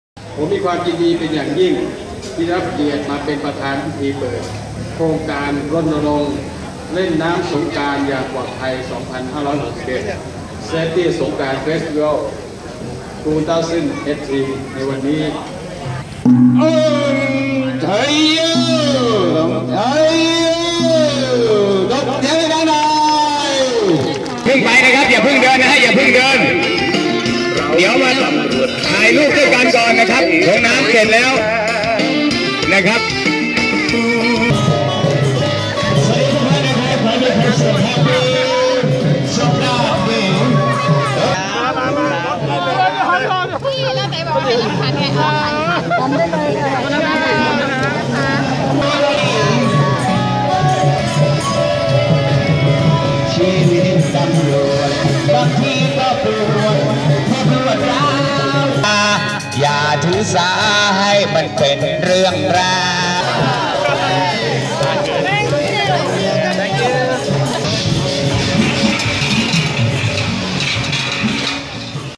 รณรงค์เล่นน้ำสงกรานต์อย่างปลอดภัย 2561 ถนนข้าวสาร
พล.ต.อ.เฉลิมเกียรติ ศรีวรขาน รอง ผบ.ตร.(ปป.) เป็นประธานพิธีเปิดโครงการรณรงค์เล่นน้ำสงกรานต์อย่างปลอดภัย ( Srongkran Water Festival Campaign Safe 2018 " ณ บริเวณ สน.ชนะสงครามและ ถนนข้าวสาร โดยมี พล.ต.ท.ชาญเทพ เสสะเวช ผบช.น. , พล.ต.ต.ภาณุรัตน์ หลักบุญ รอง ผบช.น. และ พล.ต.ต.เสนิต สำราญสำรวจกิจ ผบก.น.1 เข้าร่วมพิธี หลังจากนั้นได้เดินรณรงค์เล่นน้ำสงกรานต์อย่างปลอดภัย 2561 จากหัวถนนข้าวสาร ถึงท้ายถนนข้าวสาร 30/03/61